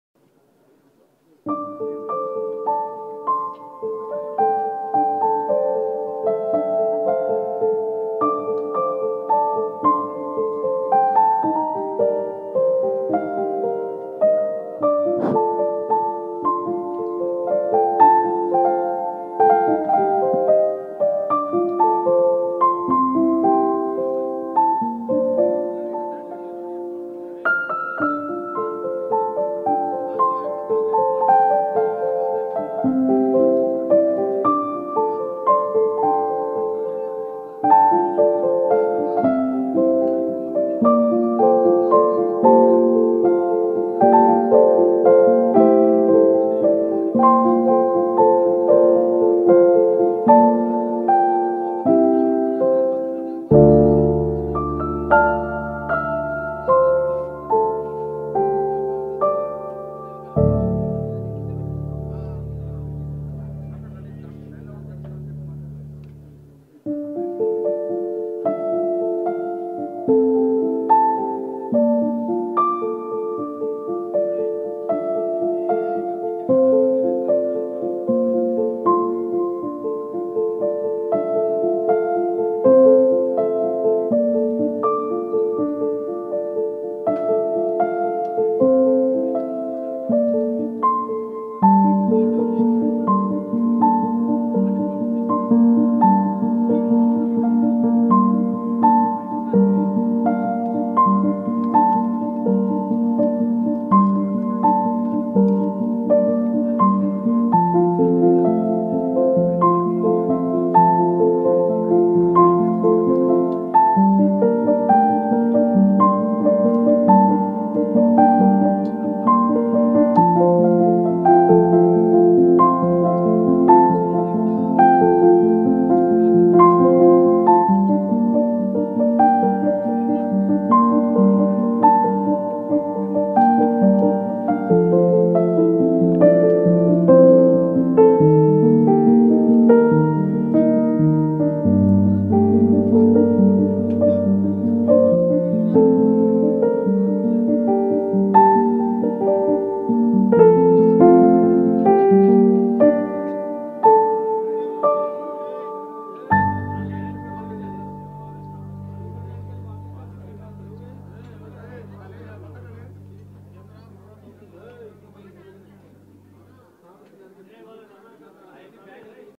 కృష్ణకు నివాళులర్పించిన బాబు | Chandrababu Pays Tribute to Super Star Krishna | Mahesh Babu | WP
కృష్ణకు నివాళులర్పించిన బాబు | Chandrababu Pays Tribute to SuperStarKrishna